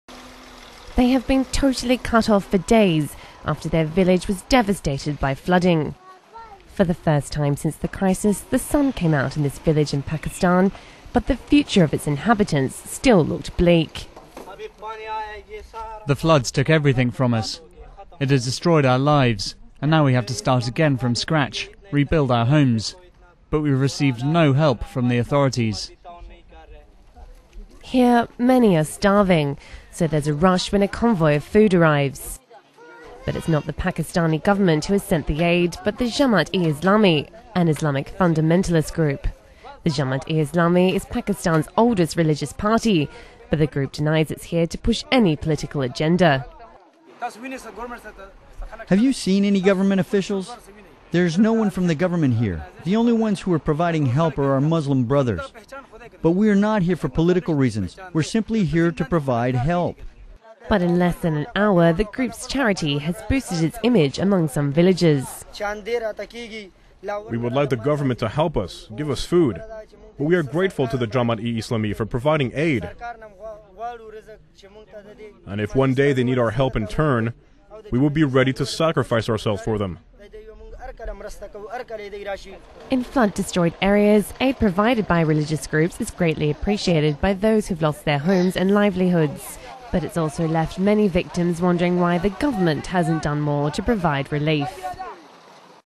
PAKISTAN: Islamists fill aid vacuum while government struggles
EXCLUSIVE - PAKISTAN: Islamists linked to the Pakistani Taliban are working hard to provide aid to their countrymen stricken by last week's devastating flooding, filling a vacuum left by government authorities.